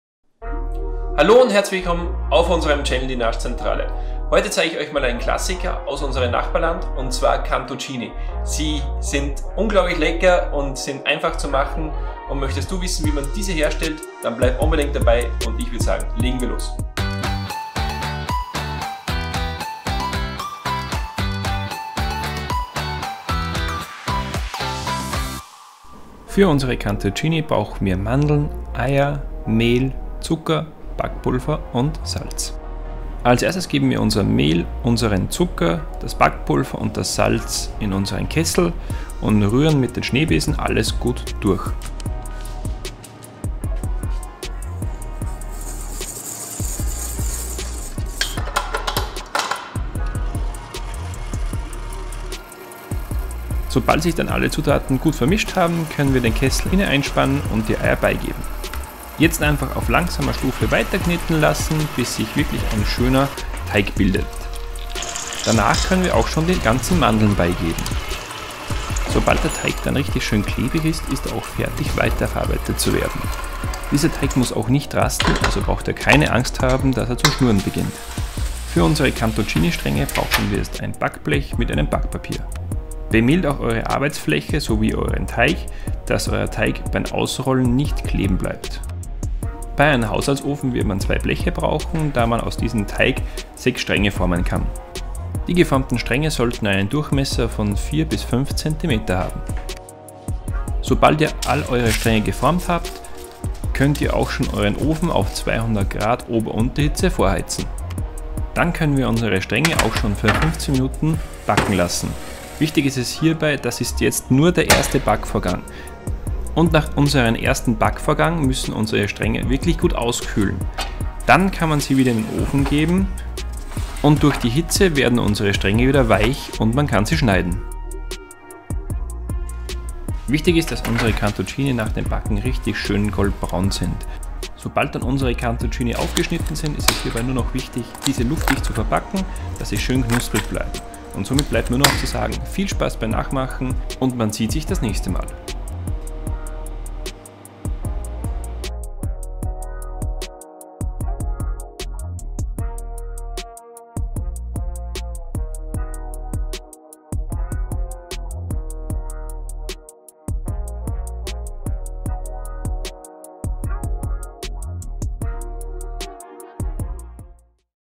CANTUCCINI Rezept! CANTUCCINI selber machen!
Werkzeug
Kenwood Cooking Chef
Konditormesser, Edelstahl
Silikon Backmatten
Silikon Teigschaber
Tortenring 24cm

das brauchst du für deine Cantuccini
200g Mandeln
400g Mehl
1 Prise Salz
1TL Backpulver
250g Zucker
4st Eier

Legen wir los!
Für den Teig gibst du Mehl, Zucker, Salz und Backpulver in deinen Kessel und mischt alles gut durch.

Sobald alles gut vermischt ist kannst du auch die Dotter beigeben und bei langsamer Stufe kneten lassen. Nach 2-3min kannst du auch die Mandeln beigeben.

Den fertigen Teig, kannst du nun in 6 gleich große Stücke einteilen und auf ein Backpapier wegsetzen (nicht mehr als 3 Stränge je Backblech). Deine Stränge sollten hierbei nicht breiter als 4-5cm sein.

Heize nun deinen Ofen auf 200C° Ober- Unterhitze vor und backe deine Cantuccini für 15-20min. Diese sollten nach dem ersten Backvorgang schon goldbraun sein. Lass jetzt deine Cantuccini für 1-2h gut auskühlen, dann gib sie wiederum ein zweites Mal in den Ofen und nach 3-5min sollten die Cantuccini wieder weich werden. Jetzt nur noch mit einem glatten Messer schräg schneiden und auskühlen lassen.

So sind auch deine selbstgemachten Cantuccini fertig, am besten halten diese in einer geschlossenen Dose.

Wir wünschen euch viel Spaß beim Nachmachen und man sieht sich beim nächsten Video. LG eure NaschZentrale